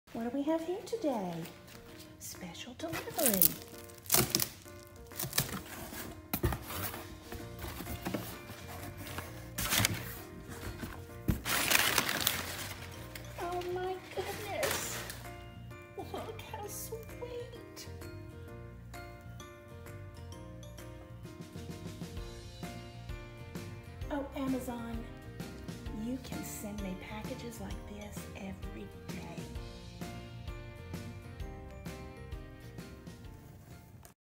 Special Delivery Box of Puppies
Who doesn’t like getting packages every day? These sweet little golden retriever puppies are just a few days old and although they did not actually arrive in a box at our front door, wouldn’t life be perfect if we could just add a box of puppies to our online shopping carts? Sign me up for auto-ship!